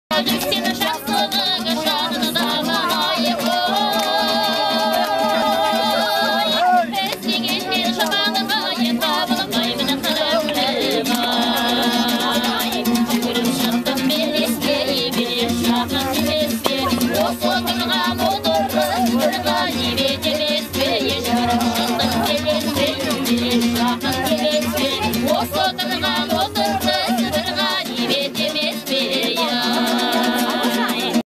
A Traditional Kazakh Duet
In the yurt again for the Naurys celebration. A jaunty little Kazakh tune.